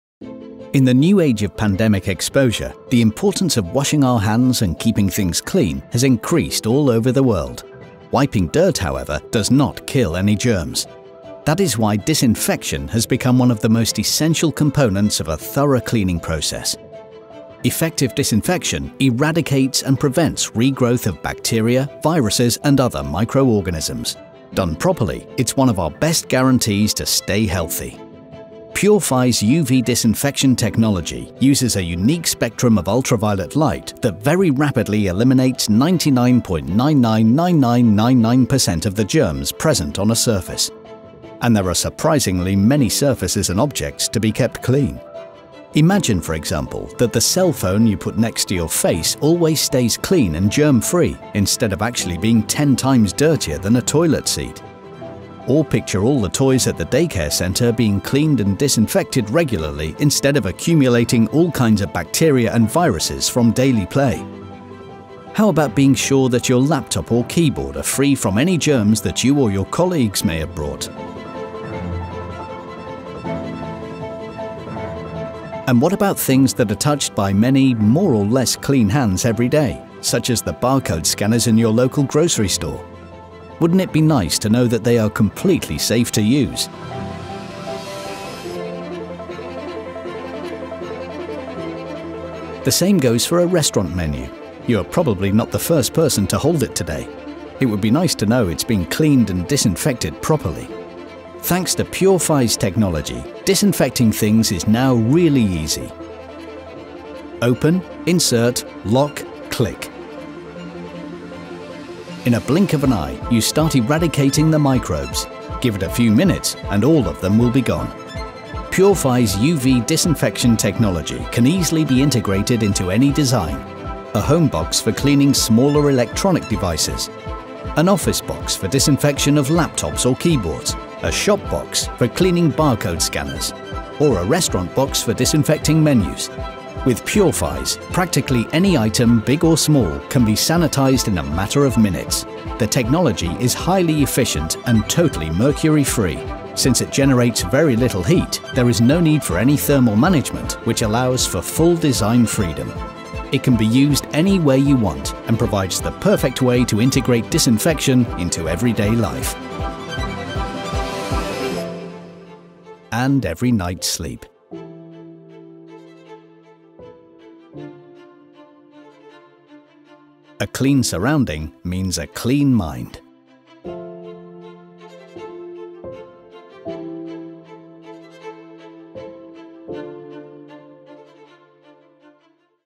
PureFize® – UV disinfection at its best
PureFize® by LightLab Sweden AB is a new UVC disinfection technology that rapidly and efficiently destroys all bacteria, viruses and other germs present on surfaces (incl. food) as well as in liquids and air.